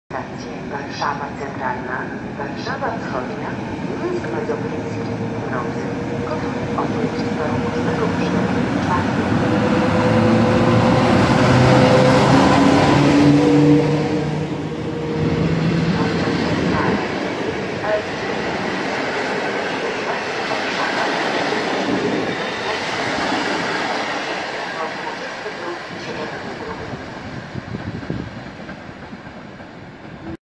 EP09-039 TLK Korczak |Warszawa Zachodnia
TLK13103 KORCZAK relacji  Warszawa Wschodnia-Kraków Główny.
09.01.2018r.